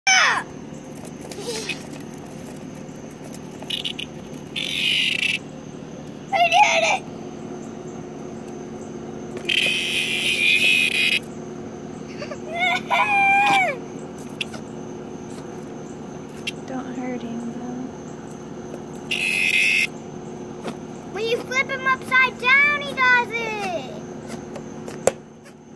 Cicada loud sound up close!
Our sons though they found the biggest fly in the world. When dad shows them what that huge fly sounds like they realize that is not a fly. So they got to learn about Cicadas. The excitement of them finding one on there own  was the best. Kids and nature go great together. Get your kids to go outside today.